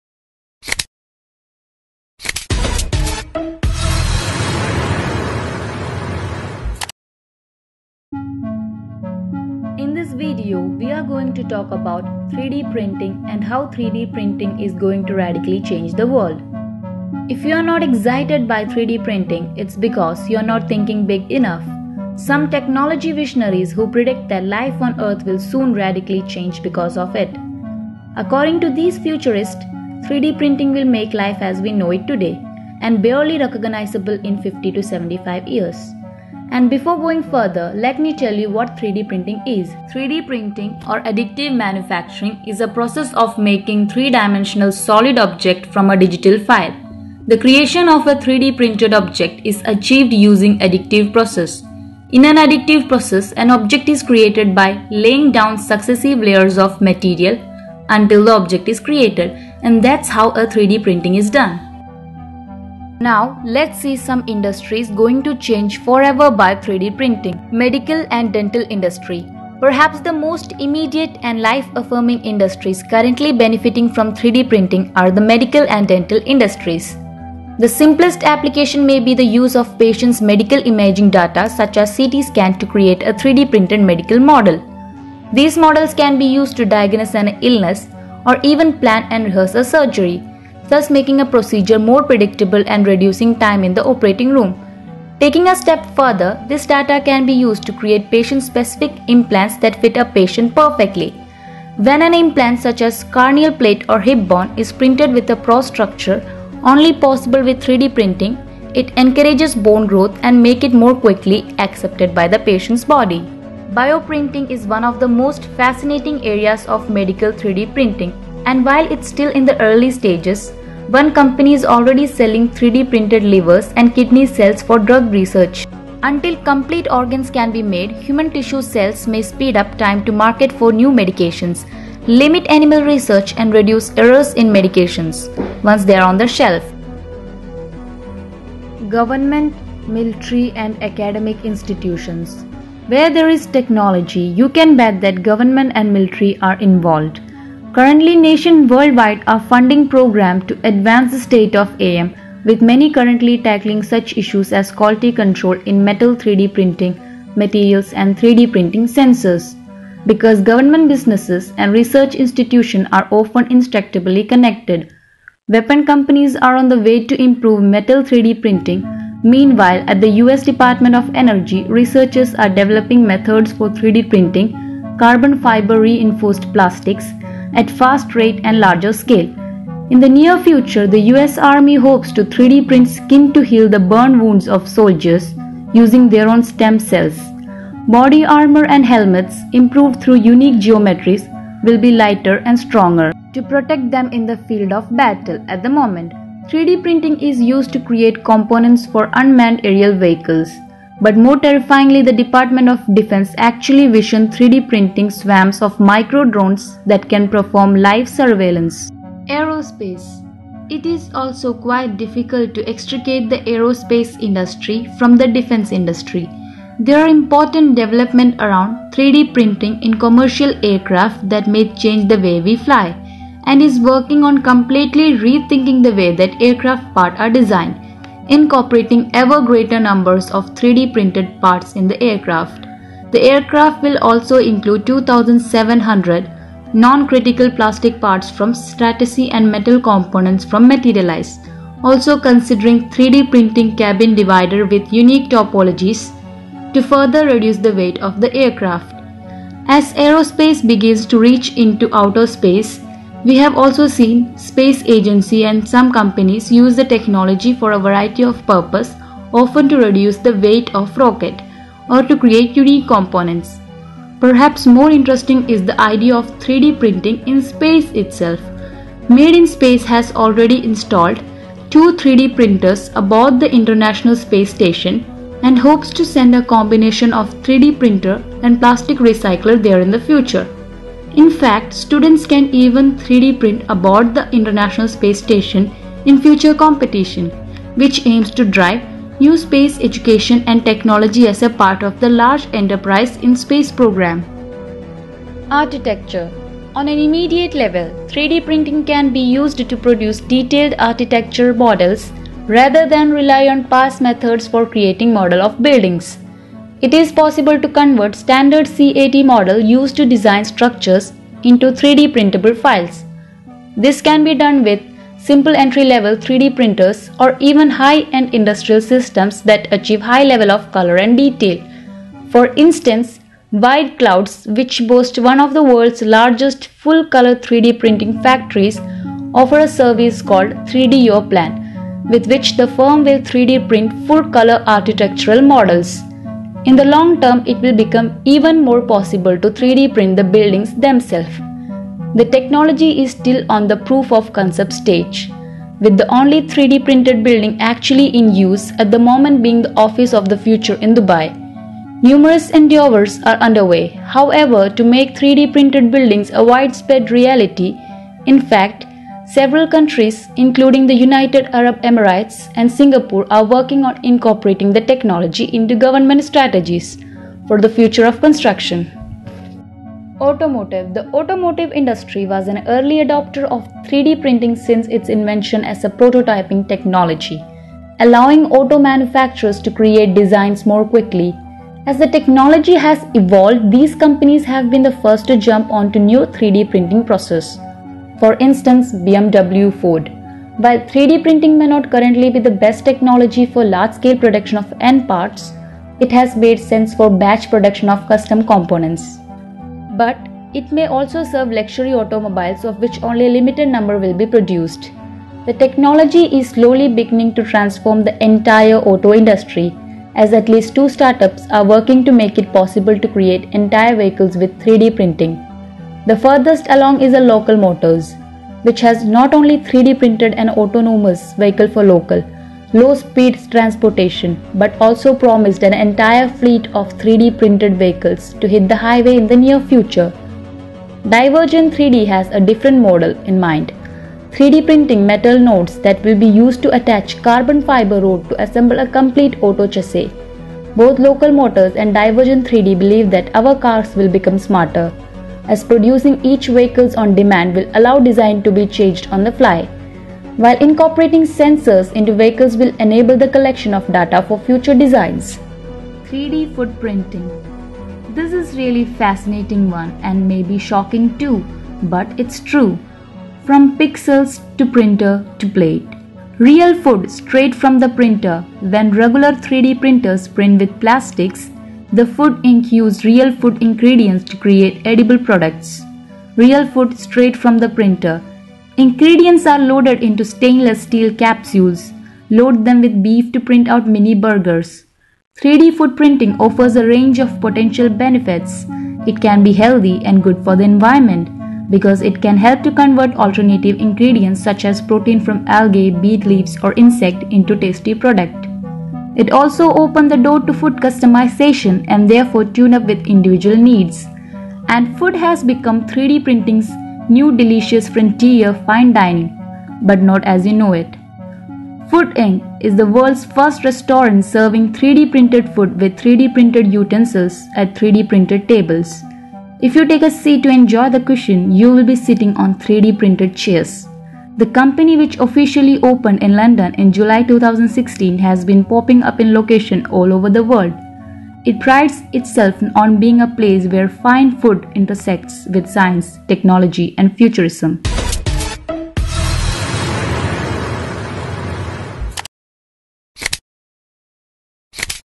Be Aware ! 3d Printing is going to Disrupt You in Every Known Ways!
The consumer 3D printing hype that promised a 3D printer in every household didn’t quite pan out, but industry use of the technology is only increasing,

additive manufacturing (AM) provides unprecedented methods for producing custom parts on demand and with complex geometries unachievable with traditional manufacturing technologies.

3D printing can be used to produce detailed architecture models. Rather than rely on past methods for creating models of buildings, it’s possible to convert standard CAD models used to design structures into 3D-printable files.